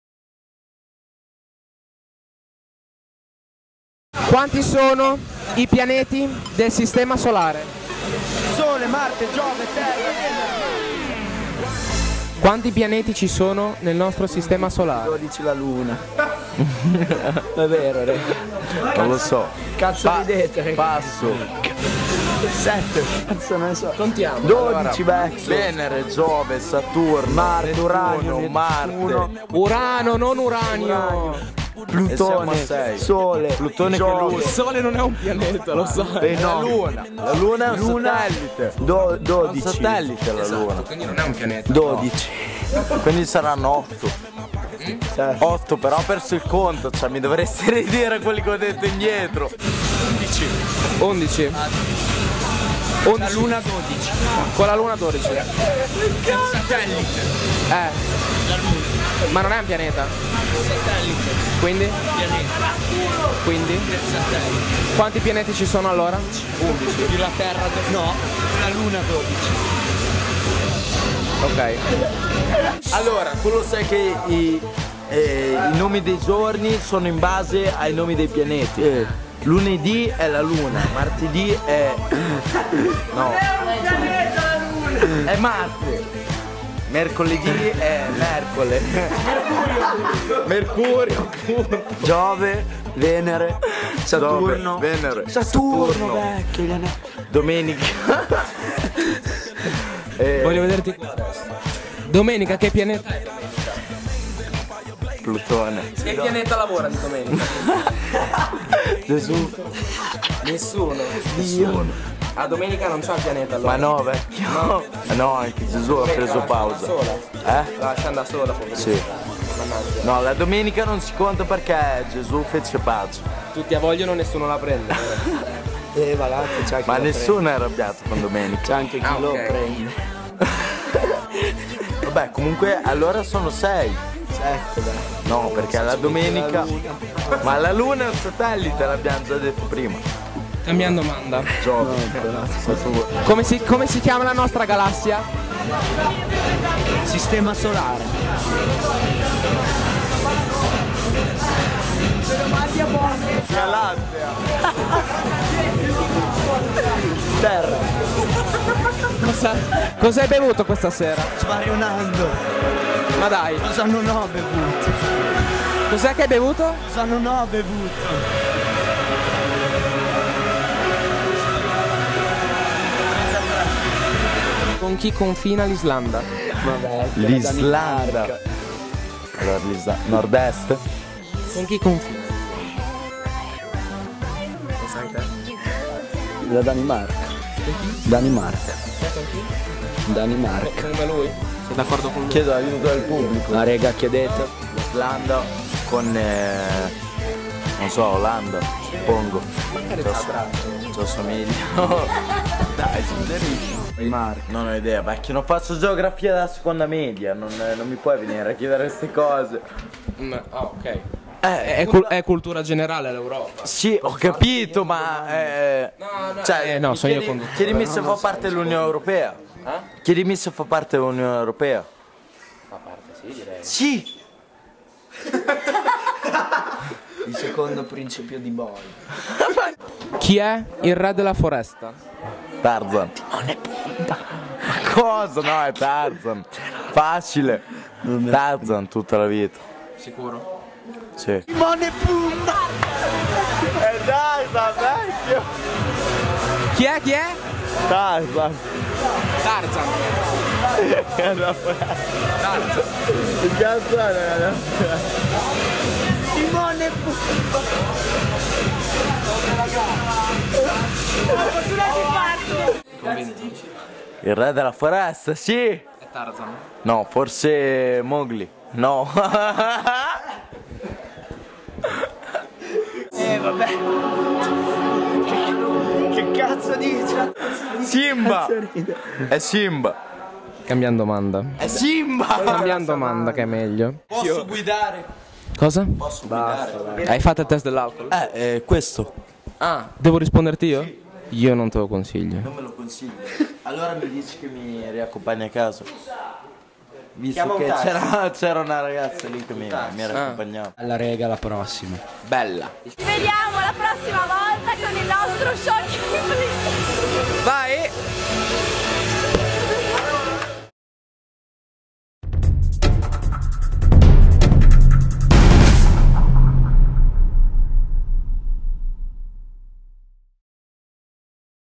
-Pier Pacio News- Shocking Quiz 2
Il nostro telegiornale scolastico è andato ad intervistare i ragazzi alla festa d'istituto,  con lo Schoking Quiz.

Festa svoltasi al Tresor, locale di Bologna.

-Pier Pacio News-